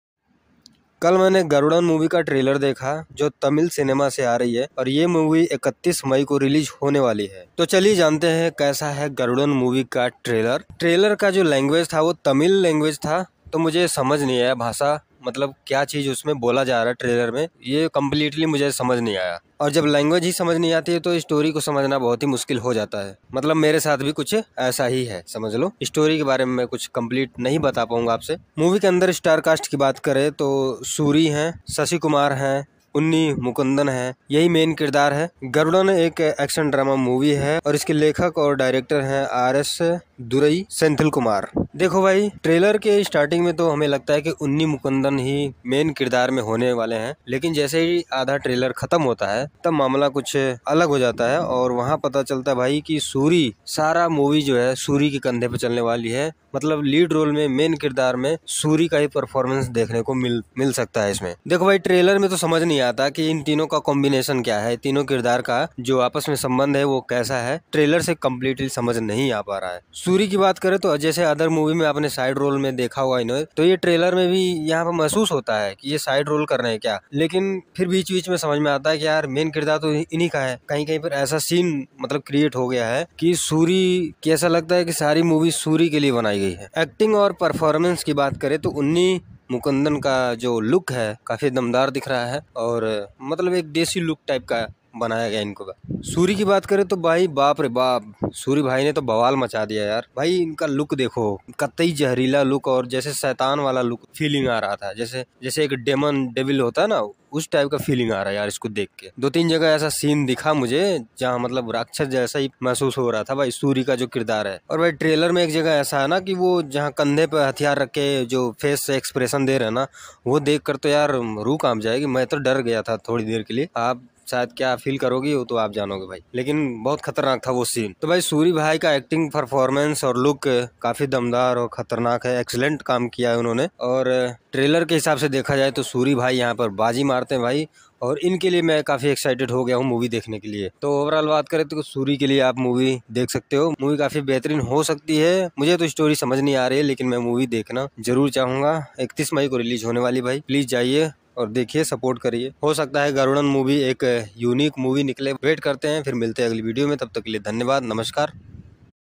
GARUDAN : Trailer review / Soori /M. Sasikumar / Unni Mukundan
Soori as Sokkan

M. Sasikumar as Aadhi

Unni Mukundan as Karuna

Revathi Sharma

Roshini Haripriyan

Samuthirakani

Garudan  is an upcoming Indian Tamil-language action drama film written and directed by R. S. Durai Senthilkumar, story by Vetrimaaran

Plot

Two childhood friends Aadhi and Kaurna had deep bond as brothers and defend each other and Sokkan was their trusted confidant whose loyal huty  become changed by dymatic into betrayal and survival.